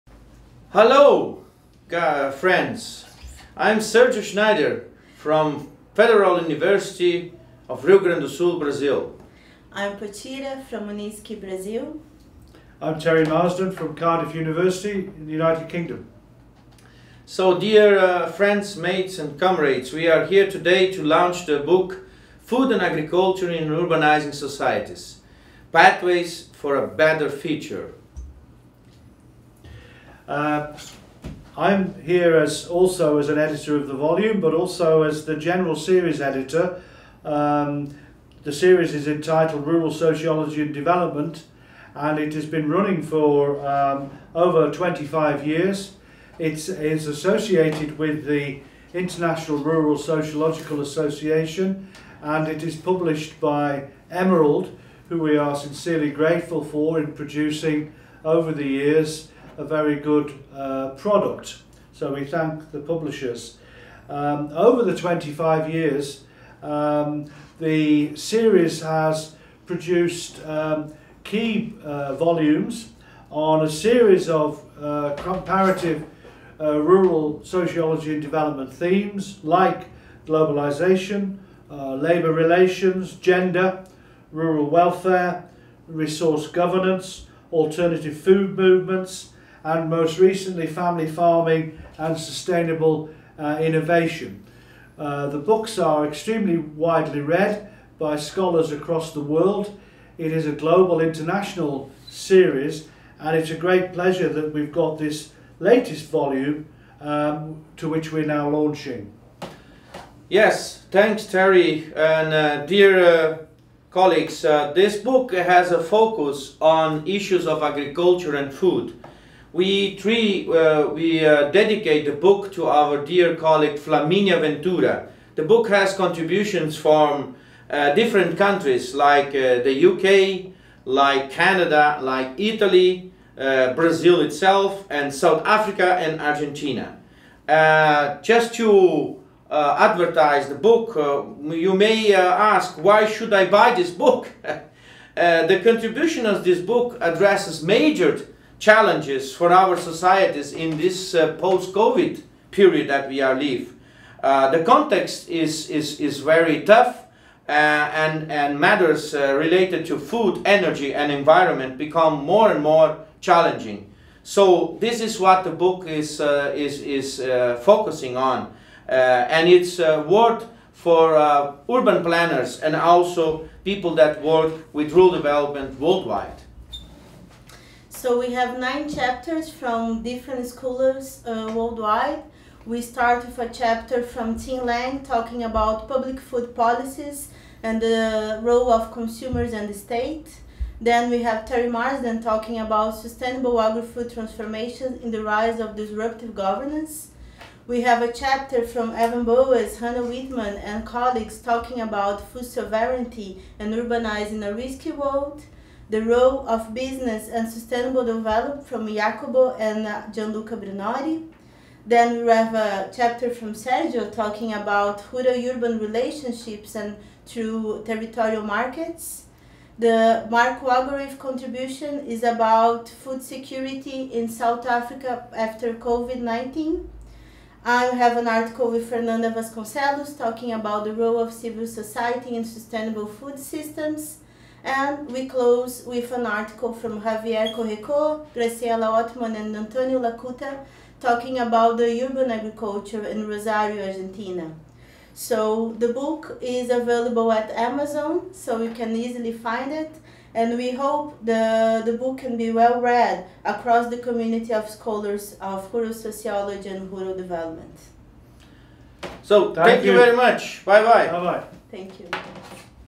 Book Food and Agriculture in Urbanized Societies: Pathways for a Better Future
It is with great satisfaction that we share with you our new book “Food and Agriculture in Urbanized Societies: Pathways for a Better Future”. Edited by Sergio Schneider, Potira V. Preiss and Terry Marsden, it is the newest volume of the Research in Rural Sociology and Development Series from Emerald Publishing.
The book has 9 chapters from leading and emerging scholars and practitioners from around the world. Besides the editors, it presents contributions from Tim Lang, Gianluca Brunori, Hannah Wittman, Marc Wegerif, Evan Bowness, Javier Couretot, Graciela Ottmann, Antonio Lattuca, among other colleagues.
It reflects on the urgency to redirect the food systems pathways, drawing from the debates set on the Third International Conference on Agriculture and Food in an Urbanized Society held on Brazil in 2018, to the striking of COVID-19 pandemic and its cascading effects to global food supply, as well as the highly rhetoric and frustrating outcomes from UN Food Systems Summit on 2021.
The publication is dedicated to Flaminia Ventura – a tenacious agrifood scholar and generous colleague, as well to all of those who spend time, energy and engagements to fight against hunger, climate change and social inequalities in these uncertain and disruptive times.
Sample chapters and purchase copies either on hardcover or ebook please go